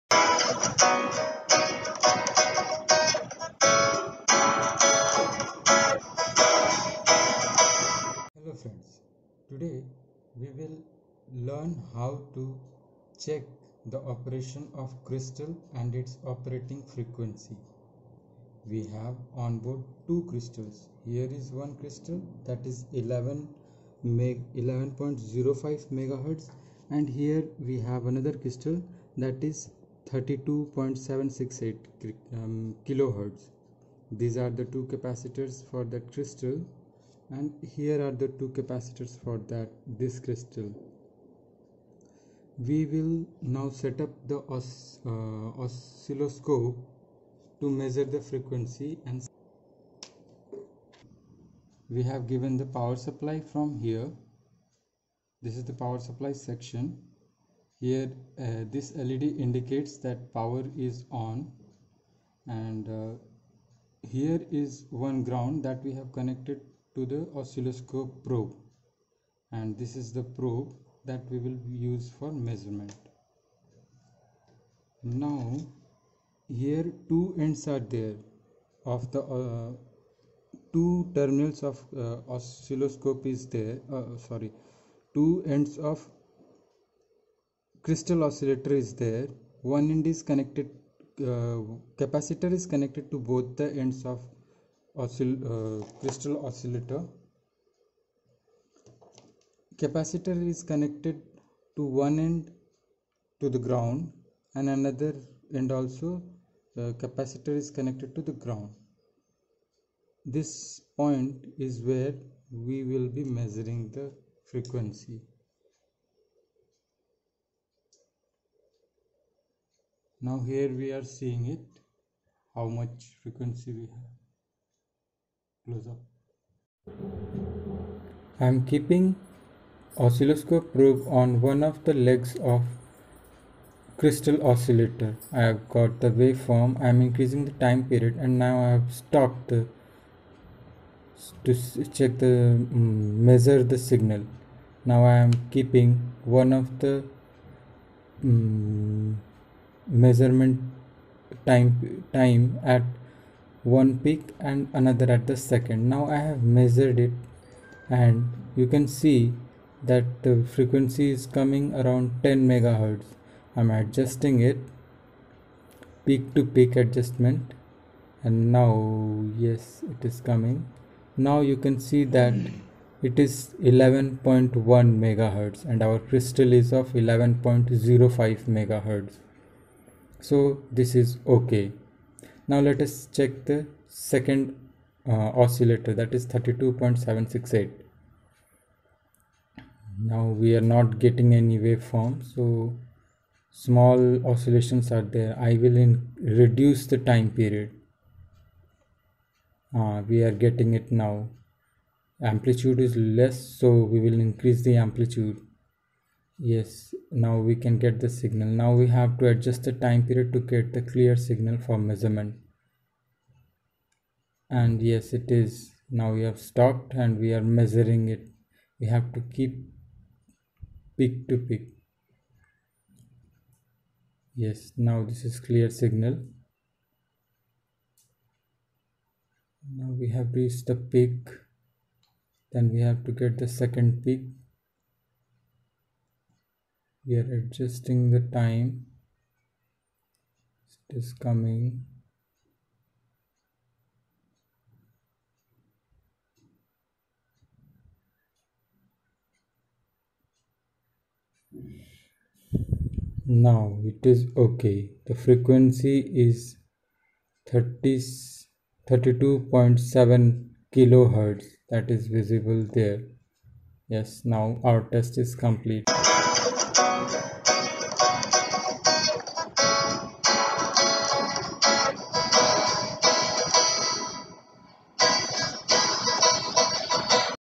Crystal Oscillator debugging with Hardware & Oscilloscope || Microcontroller Workshop Part-05
crystal Oscillator debugging with Hardware & Oscilloscope Explained
Actual Hardware board shown and explained
Waveform shown in DSO